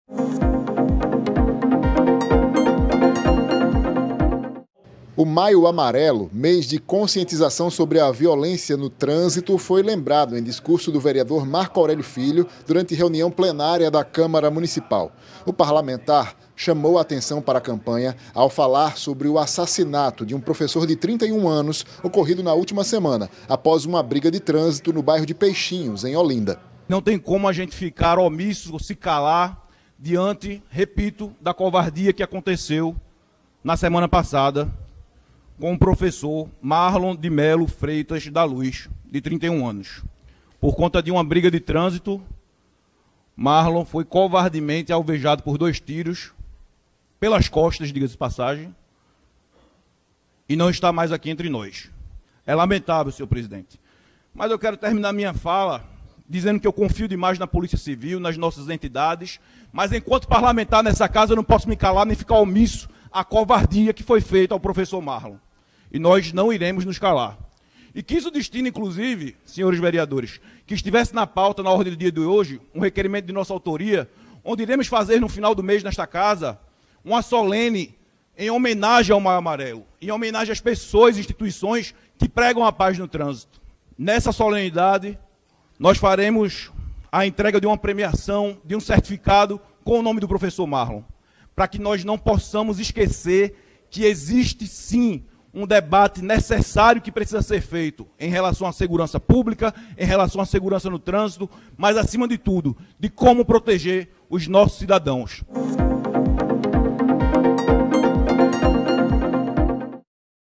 Marco Aurélio Filho lembra Maio Amarelo
O Maio Amarelo, mês de conscientização sobre a violência no trânsito, foi lembrado em discurso do vereador Marco Aurélio Filho durante reunião plenária da Câmara Municipal.